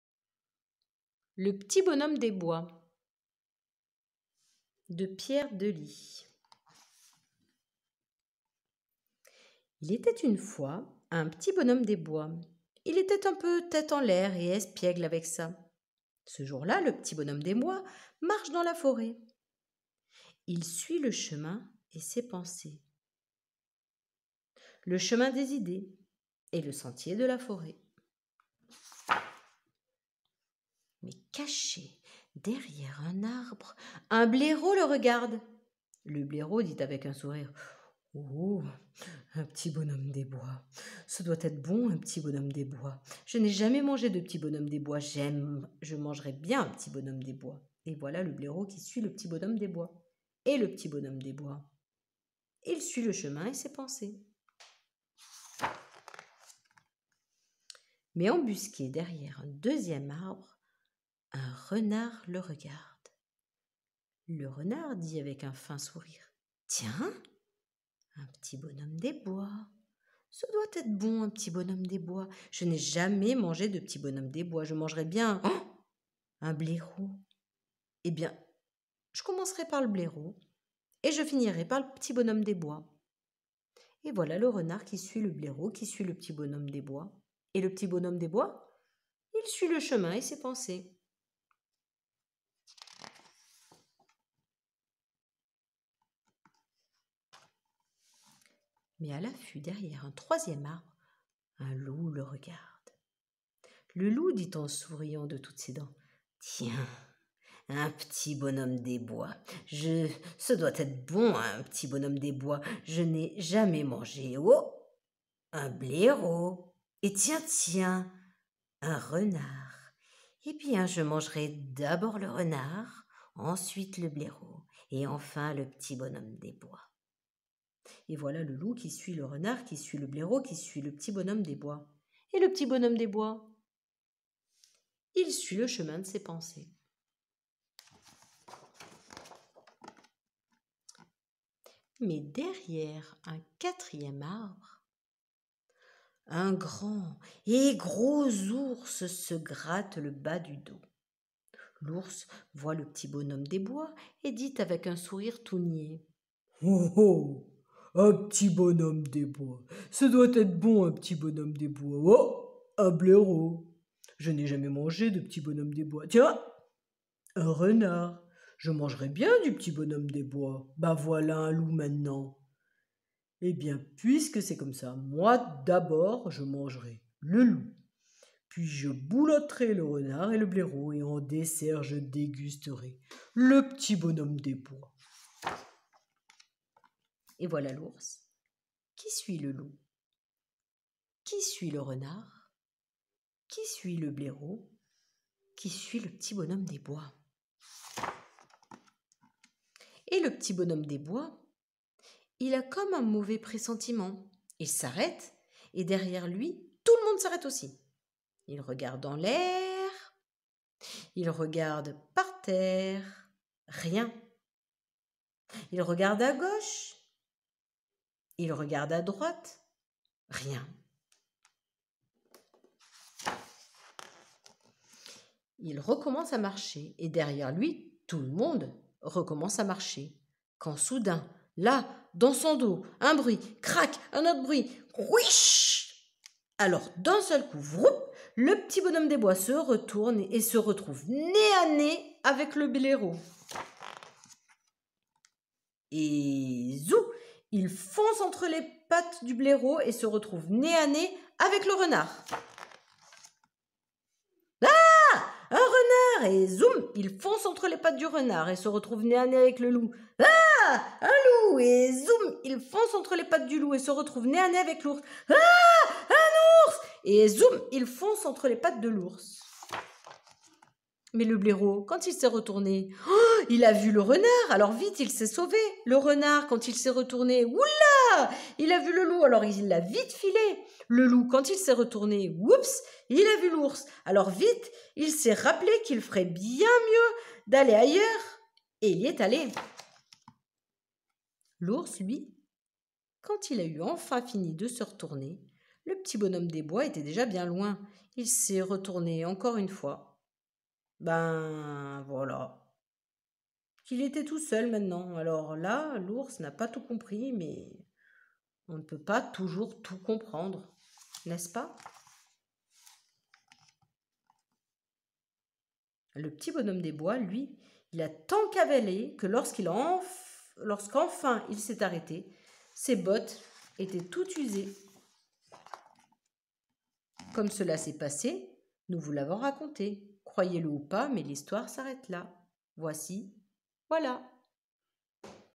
🚶‍♂️🦡🦊🐺🐻Le p'tit bonhomme des bois (de Pierre Delye) histoire racontée par maîcreeeeesse krokinette
Travail en lien avec l'orientation dans l'espace et la réécriture du livre en utilisant les pictogrammes et les chaines alimentaires.
A mettre en réseau avec d'autres histoires en randonnées.
Dans mon sac à album je mets: les pictogrammes des animaux à faire correspondre aux marottes.